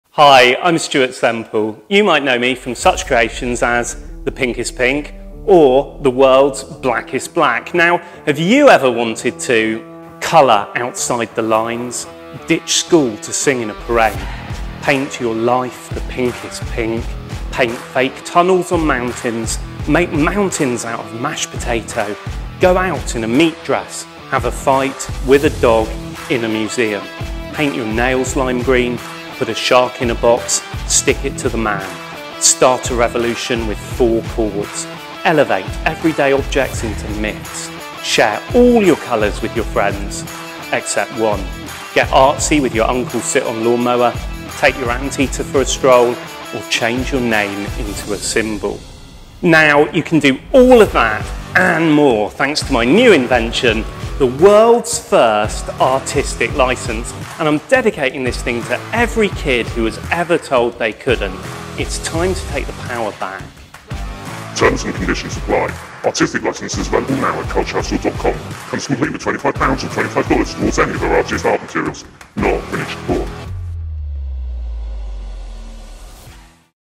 LICENSED TO ART | Just Flash Our New Artistic License
We were all born with a magical artistic license that means we can express ourselves however we want. Sadly the world has forgotten and we are victim to teachers, parents and authorities who are told us we are wrong. So we have decided to issue official artistic licenses. These cards can be carried and produced if any rotters try to scupper your creativity. They are available now from culturehustle.com and come with $25 / £25 worth of the world’s most potent art materials.